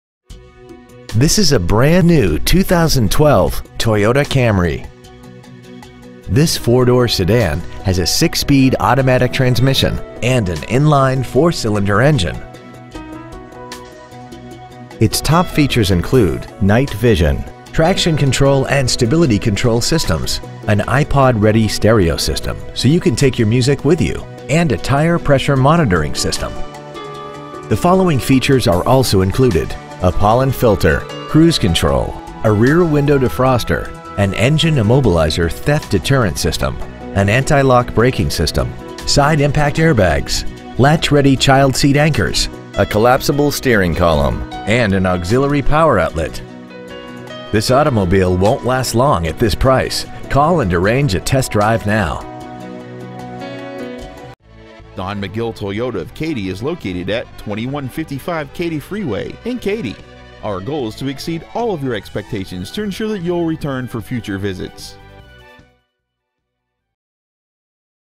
2012 Toyota Camry Katy Texas
Year : 2012
 Make :
 Model :
 Engine : Gas I4 2.5L/152
 Trans . : Automatic
 Exterior : Magnetic Gray Metallic

 Interior : Ash
 Stock : 270775

 2012 Katy Texas